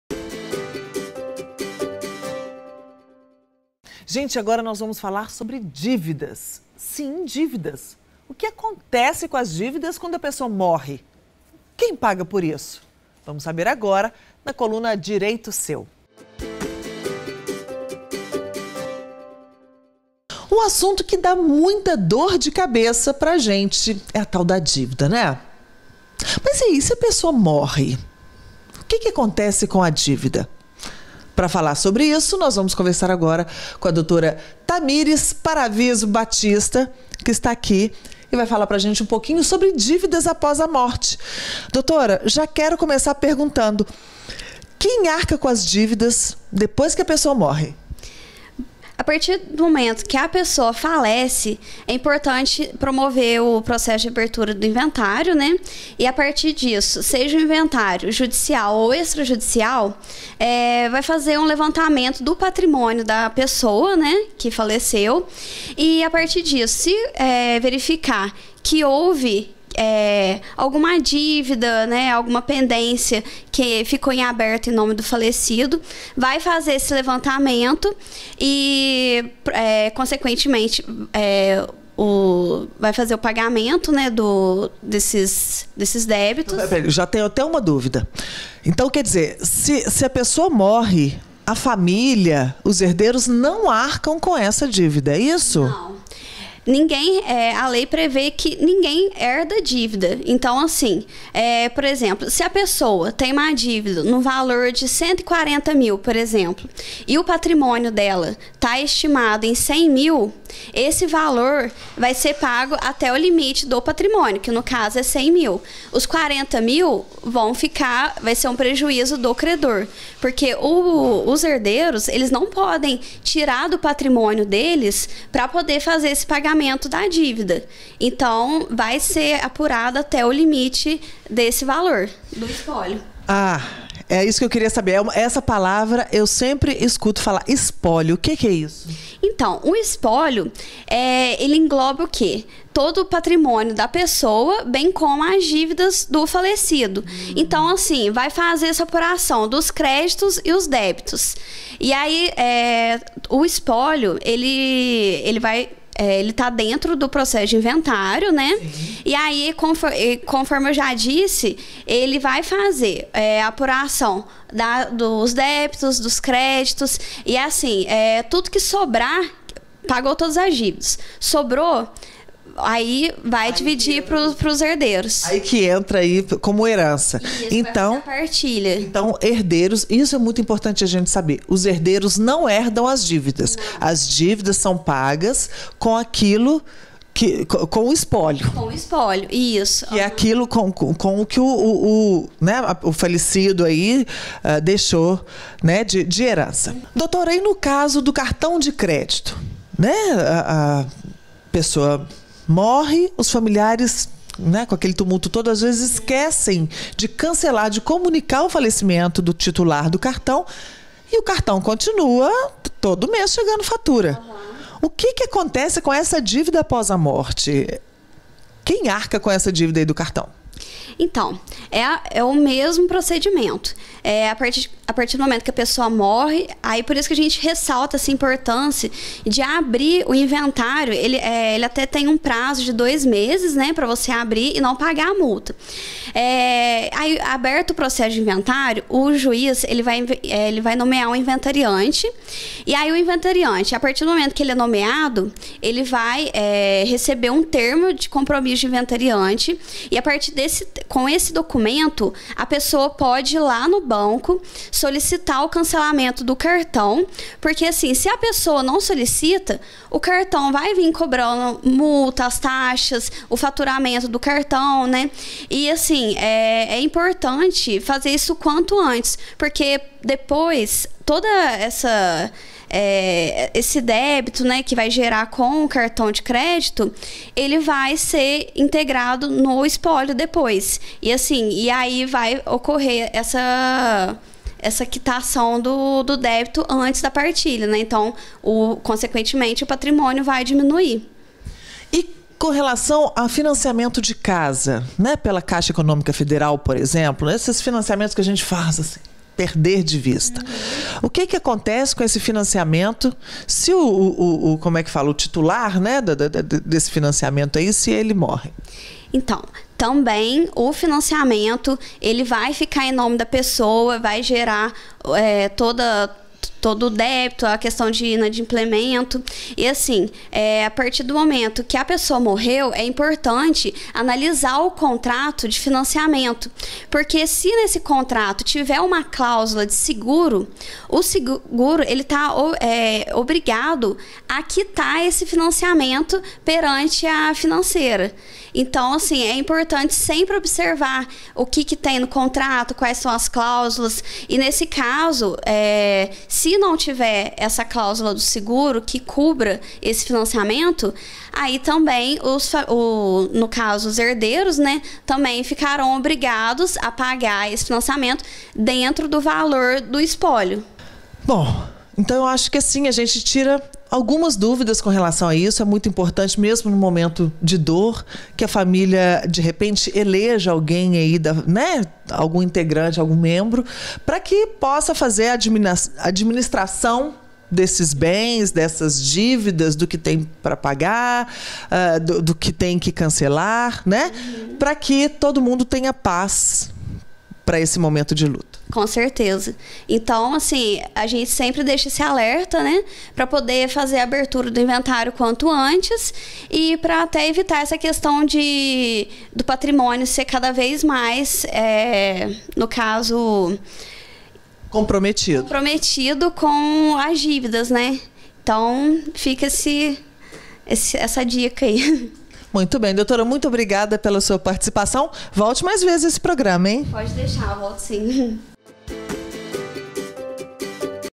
Dívidas após a morte
Saiba o que fazer com as dívidas de pessoas falecidas.